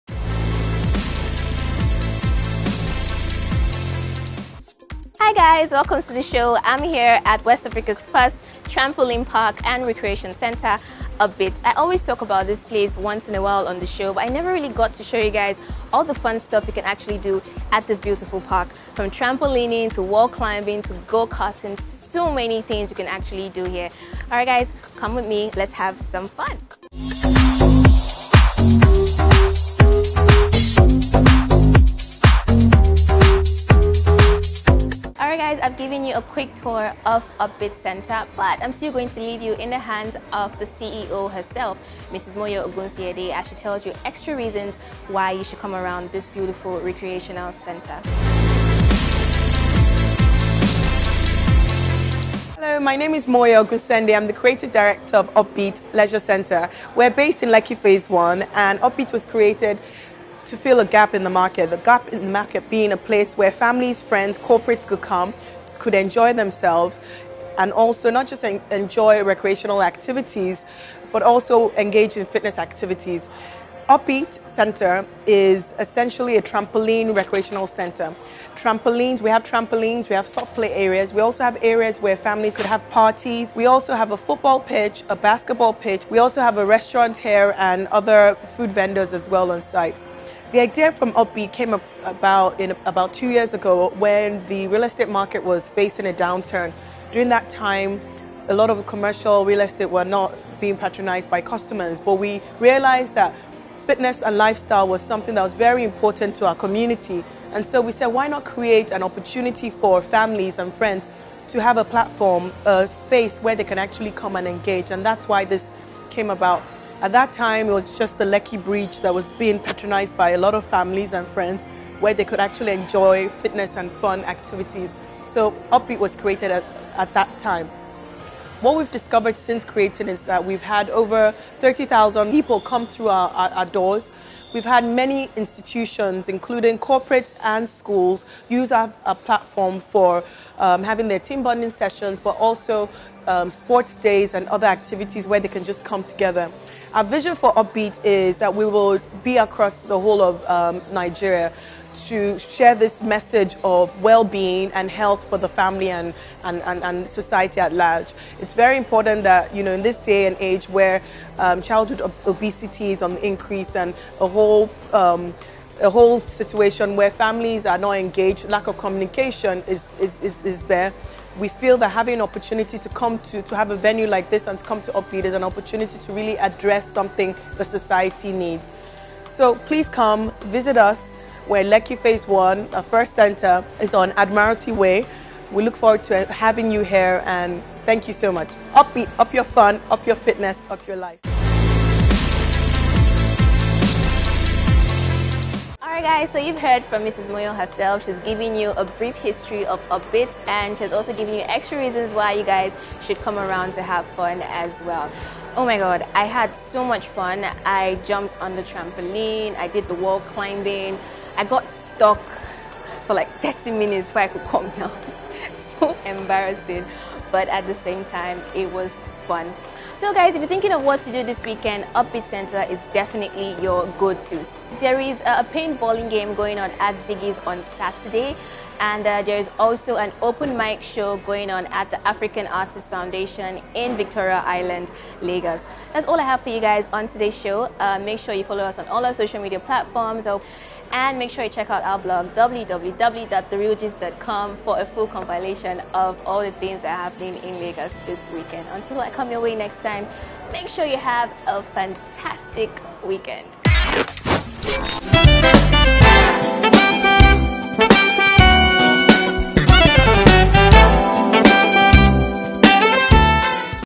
Things to do in Lagos S2E15 Trampolins in Nigeria
Do you like trampolins? Join Violet as she shares what having fun at "The Upbeat" looks like? It would be great to spend the weekend at "Upbeat".

You can even lose a pound of two, talk about weight loss without having to exercise.